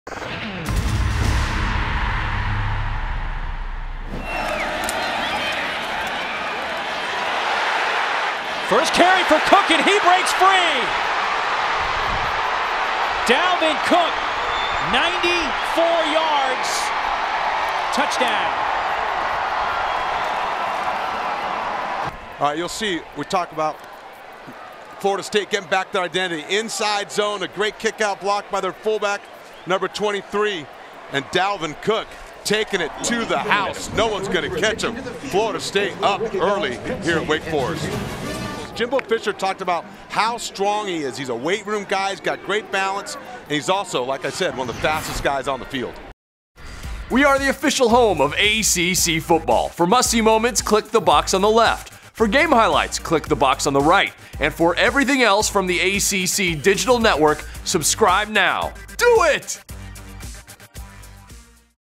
FSU's Dalvin Cook Races 94 Yards For TD | ACC Must See Moment
FSU running back Dalvin Cook takes the handoff, makes a move and races 94 yards for the first touchdown of Florida State's game against Wake Forest in this ACC Must See Moment.